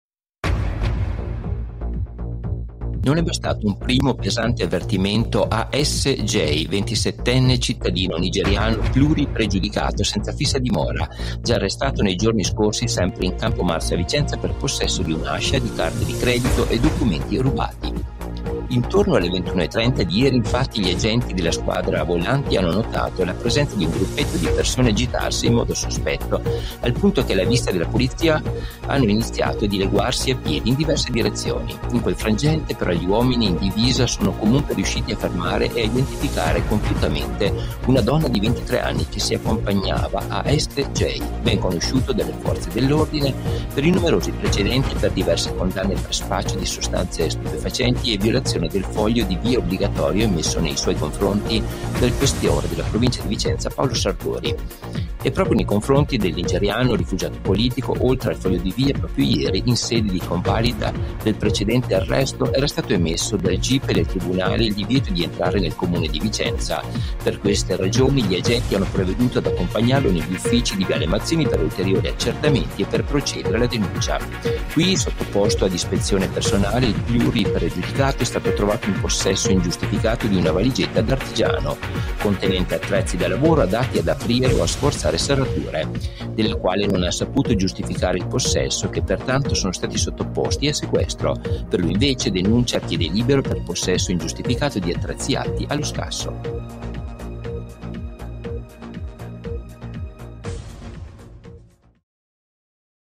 Fermato ancora dopo l’arresto con divieto di tornare in città: aveva con sè attrezzi da scasso
Non è bastato un primo pesante "avvertimento" a S. J., 27enne cittadino nigeriano, pluripregiudicato senza fissa dimora, già arrestato nei giorni scorsi sempre in Campo Marzo a Vicenza per possesso di un’ascia, di carte di credito e documenti rubati.  Intorno alle 21.30 di ieri infatti, gli agenti della della Squadra "Volanti" hanno notato la presenza di un gruppetto di persone agitarsi in modo sospetto al punto che, alla vista della Polizia, hanno di dileguarsi a piedi in diverse direzioni. In quel frangente, però, gli uomini in divisi sono comunque riusciti a fermare e ad identificare compiutamente una donna di 23 anni che si accompagnava a S. J., ben conosciuto dalle forze dell’ordine per i numerosi precedenti e per diverse condanne per spaccio di sostanze stupefacenti e violazione del Foglio di Via Obbligatorio emesso nei suoi confronti dal Questore della Provincia di Vicenza Paolo Sartori. Per l'uomo, denuncia a piede libero anche per possesso ingiustificato di attrezzi atti allo scasso.